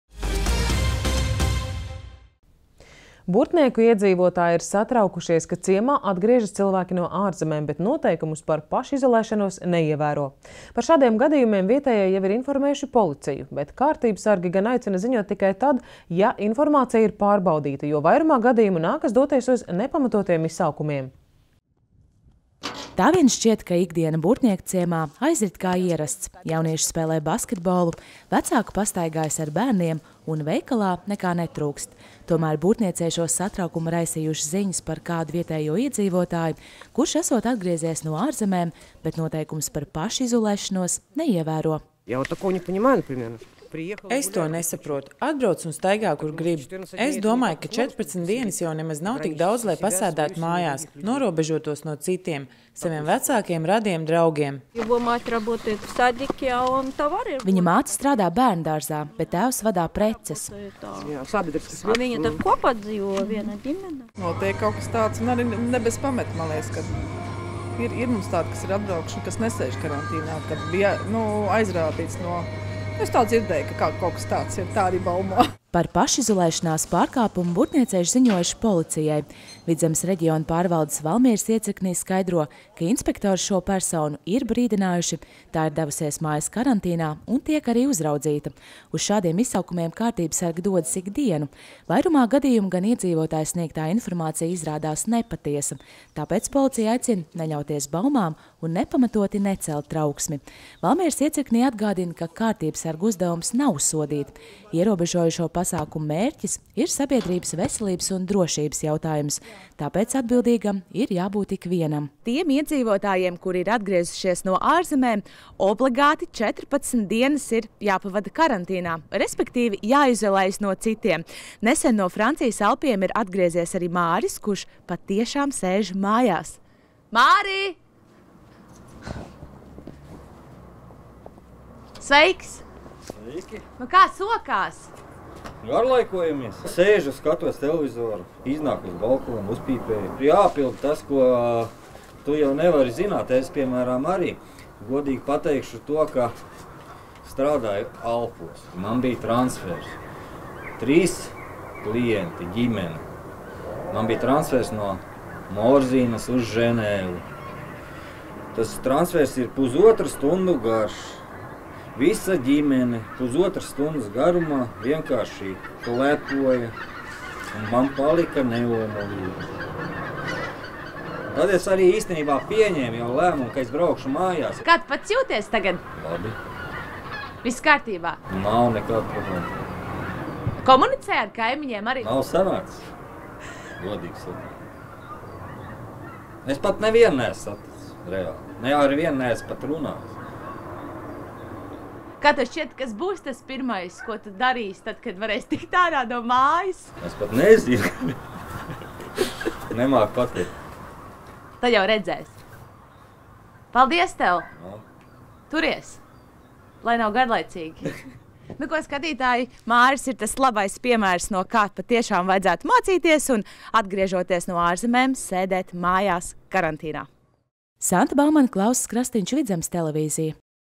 Burtniekos satraucas par pašizolēšanās noteikumu neievērošanu
Burtnieku novada iedzīvotāji nav izņēmums un arī satraucas, vai cilvēki, kuri ieradušies no ārvalstīm, apzinīgi ievēro to, ka viņiem 14 dienas jāatrodas pašizolācijā dzīvesvietā. Skaties Vidzemes TV sižetā, kā piesardzības pasākumus ievēro Burtniekos!

Burtnieku novada pašvaldības policija pirms ziņošanas par pārkāpumiem aicina rūpīgi pārbaudīt, vai tiešām cilvēkam ir jāievēro īpaši piesardzības pasākumi. Tāpat aicina neievietot nepārbaudītas ziņas par līdzcilvēkiem savos sociālo tīklu profilos! Par pašizolācijas un karantīnas pārkāpumiem un sabiedrisku pasākumu neatļautu rīkošanu ārkārtas situācijā var zvanīt uz 110!